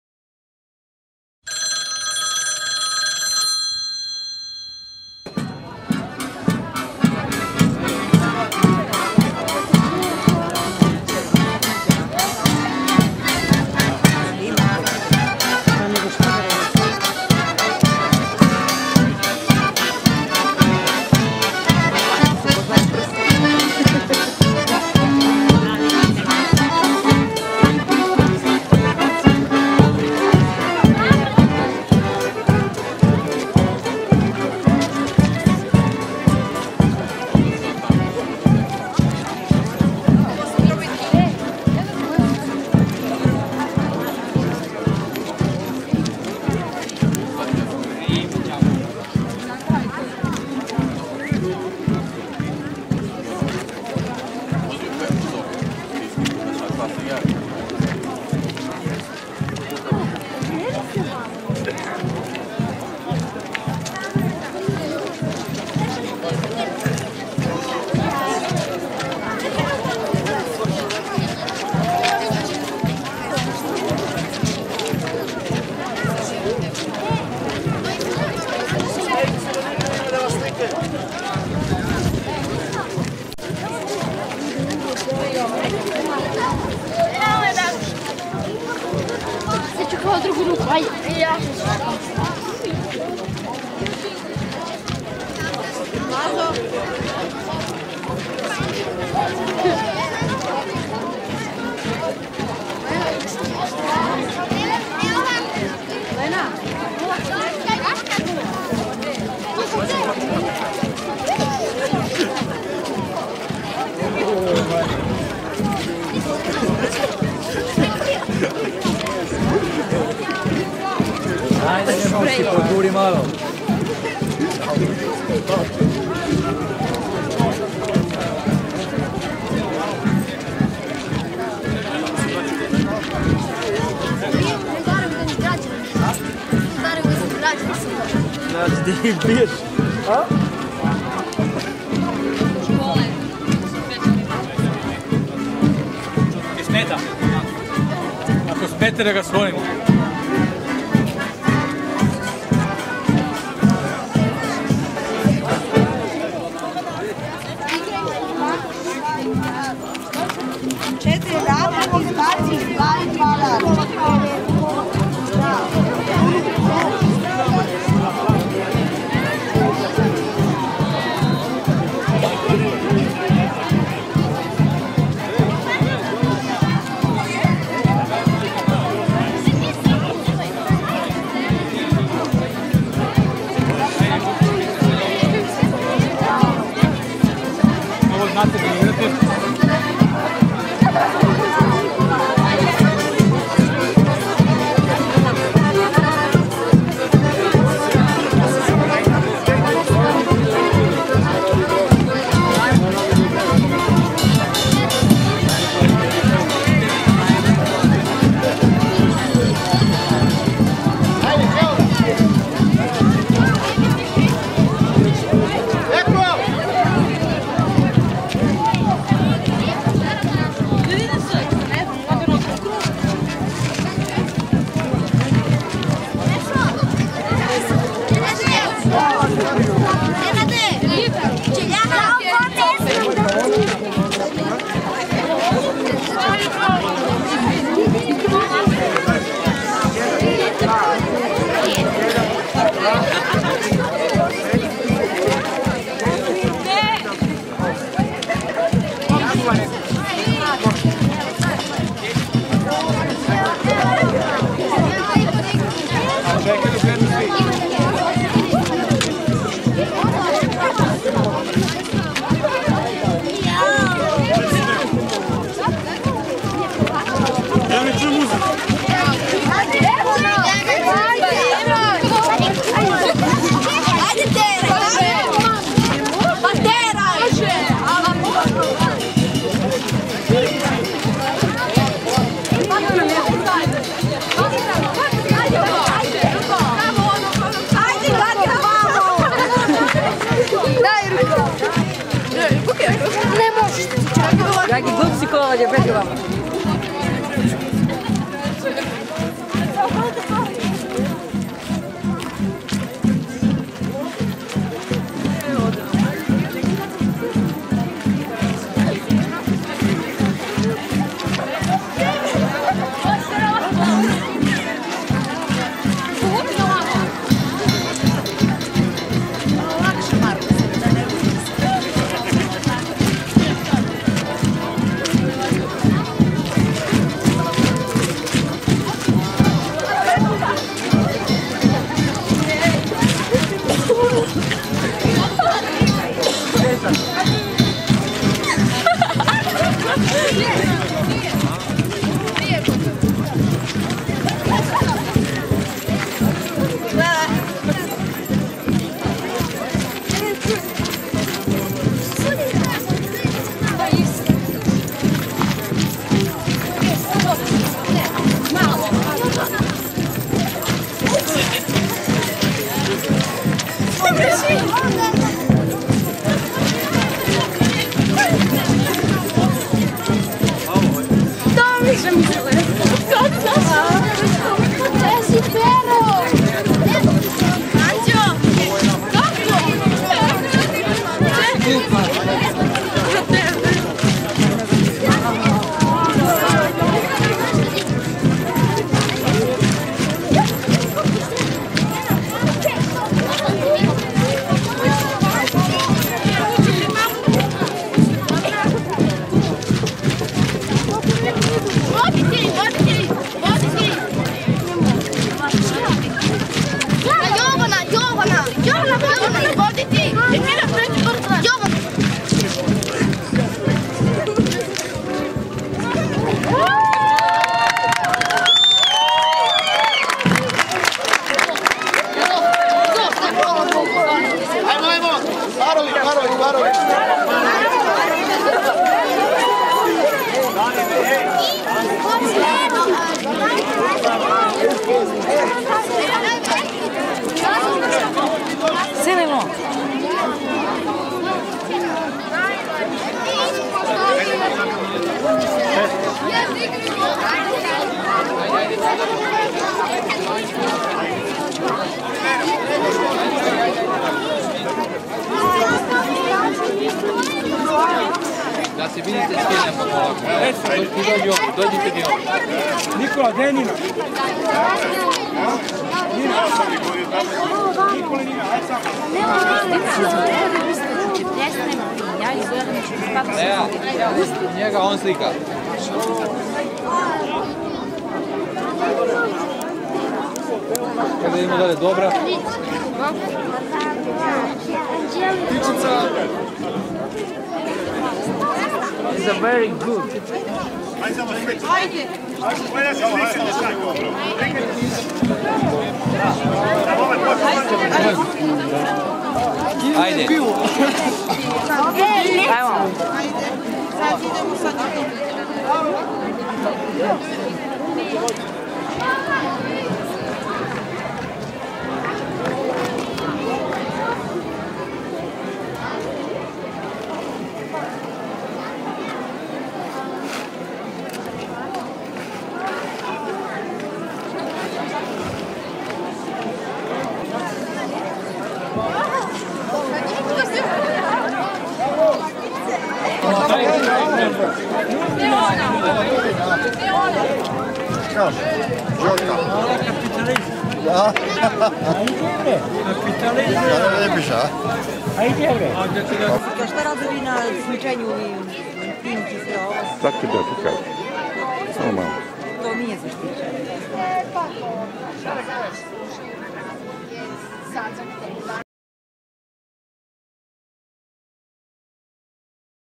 19. "DENS" Gradskovo 1.
19.Festival izvornog dečijeg narodnog stvaralaštva "DENS" održao se 4.aprila 2015 godine u selu Gradskovo. Domaćini su i ovoga puta ugostili dosta folklornih grupa Opštine Zaječar, čak 12 grupa. Festival se održao u Domu kulture u Gradskovu.